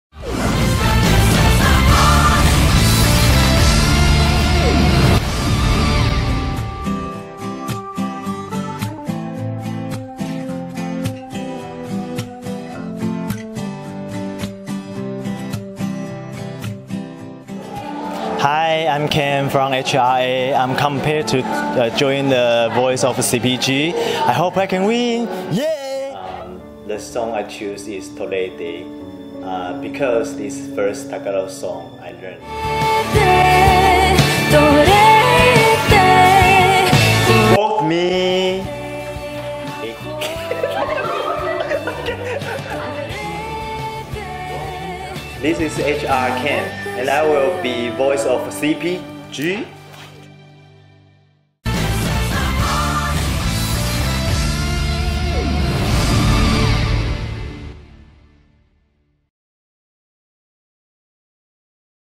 CPG VOICE: KEN CHANG INTRO v2
This video was created for CPG's Mid-autumn social event happened on Sept. 14 at Okada Manila.